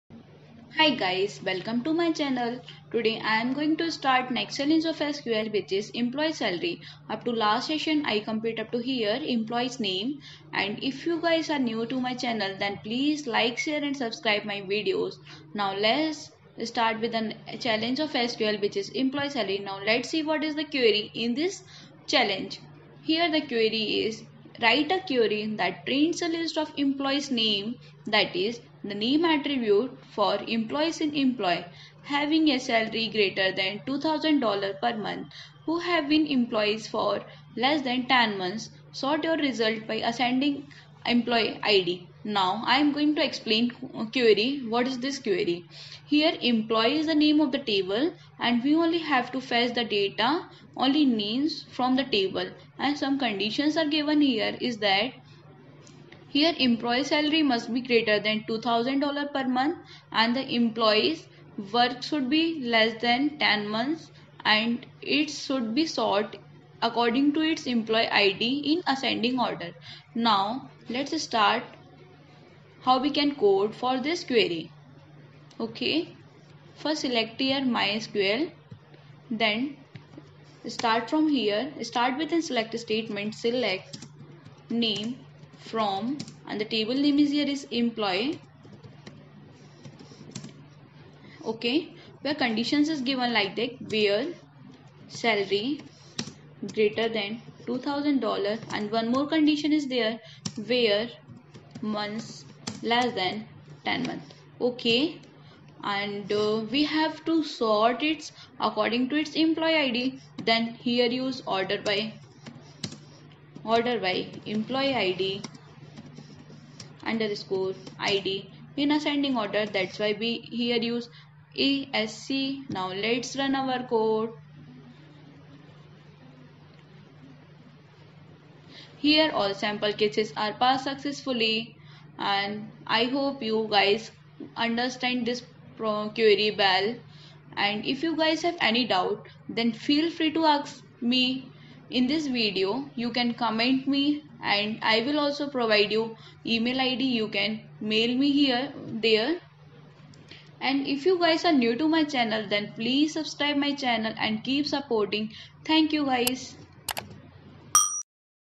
HackerRank Practice Sql 20 | Employee Salaries
Hey guys ,

In this video,I have explained HackerRank SQL challenge Employeesalaries step by step with an easy explaination.